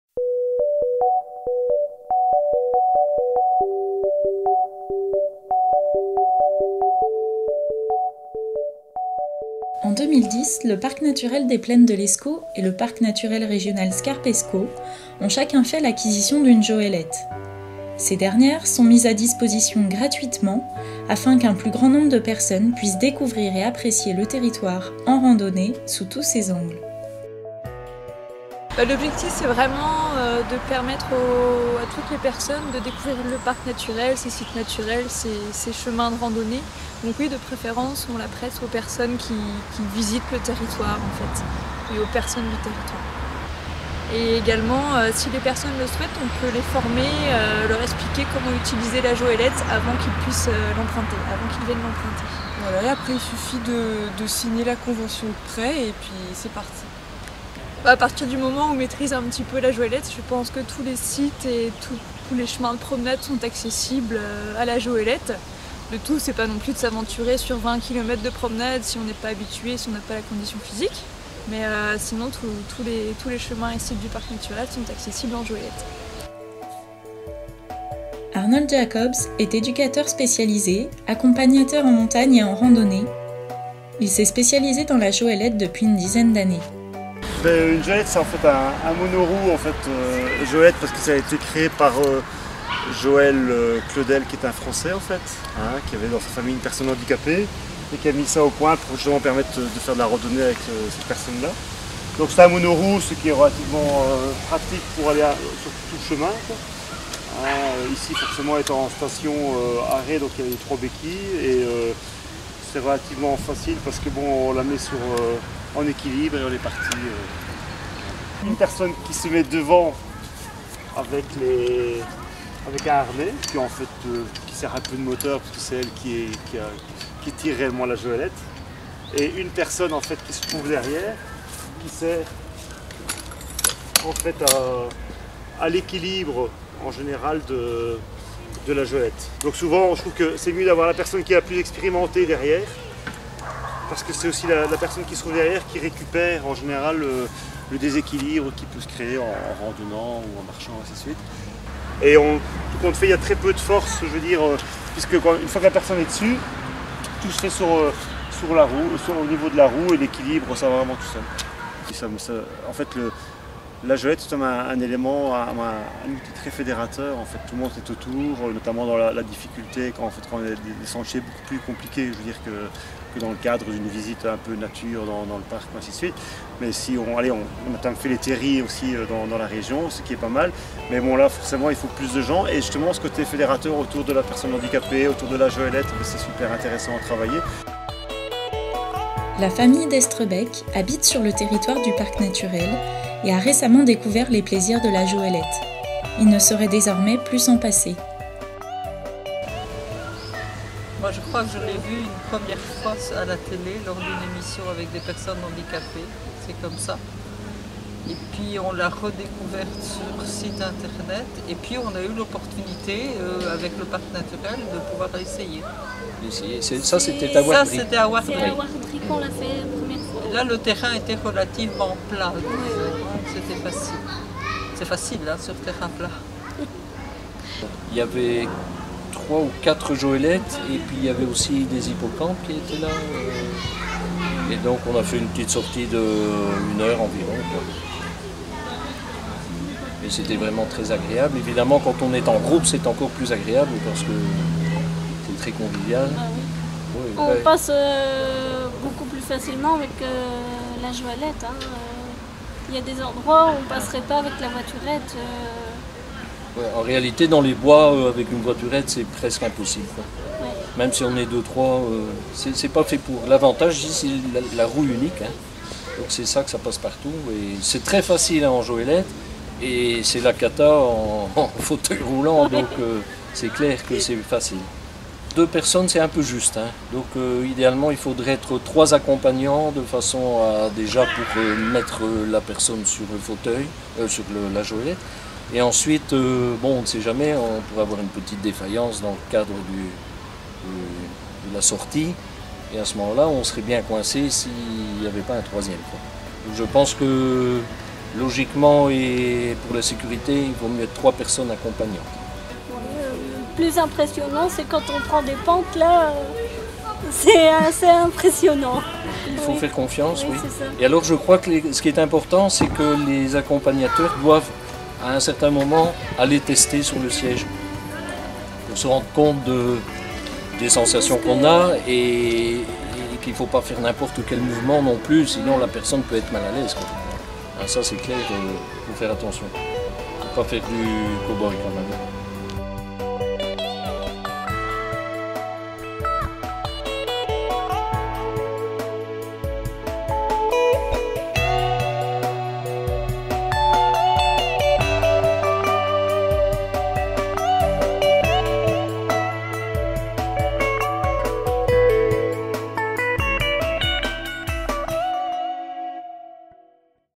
Montage des joelettes en Prêt au Parc naturel
Cet équipement permet à une personne à mobilité réduite (âge avancé ou difficultés physiques, handicap) de partir en randonnée sur tout chemin, accompagnée par deux personnes (une devant, une derrière). Ce "fauteuil roulant" est prêté aux communes, associations et particuliers (caution de 200 euros).